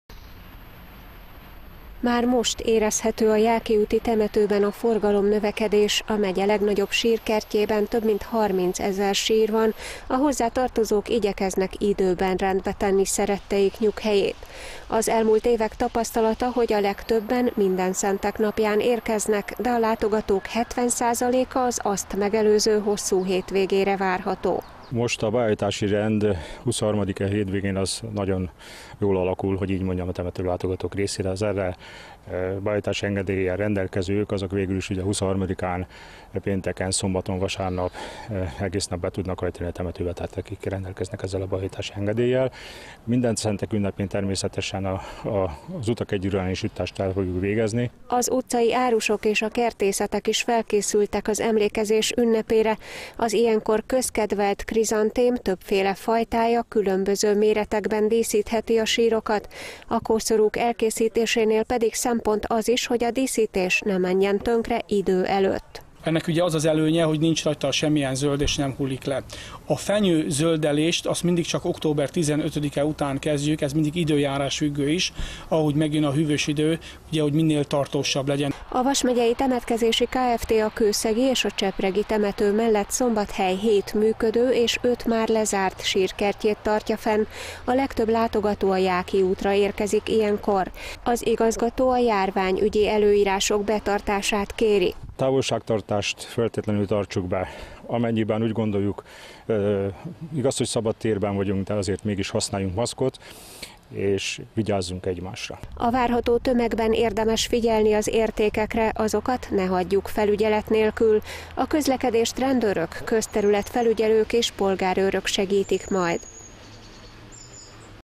A hosszú hétvégén már roham várható a temetőkben
A mindenszenteket megelőző napokban tízezrek kelnek útra, hogy rendbe tegyék szeretteik sírjait, és elhelyezzék az emlékezés virágait. A leglátogatottabb Jáki úti temetőbe ezen a héten még korlátozás nélkül behajthatnak azok, akiknek erre engedélyük van.